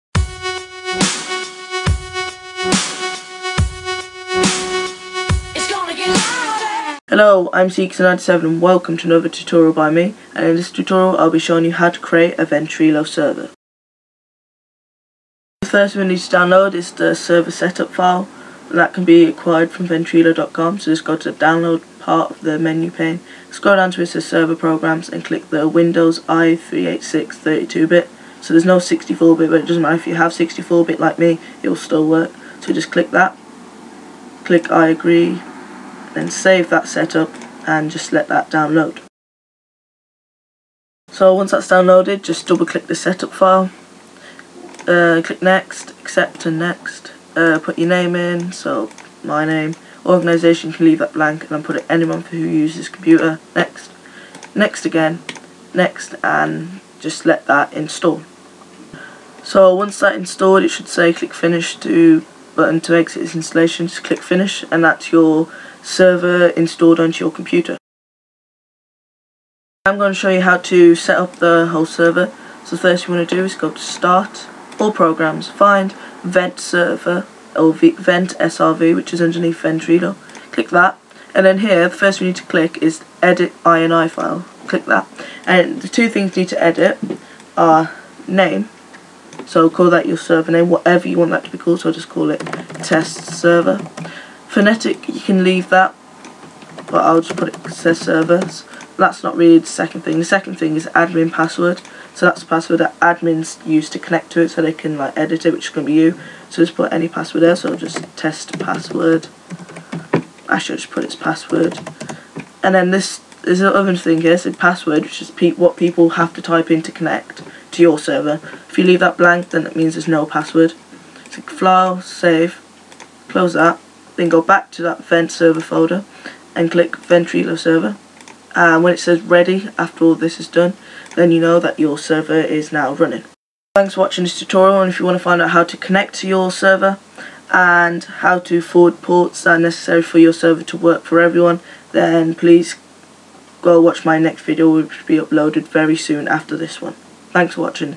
Ventrilo / How To Create A Server
show you how to setup a server for ventrilo.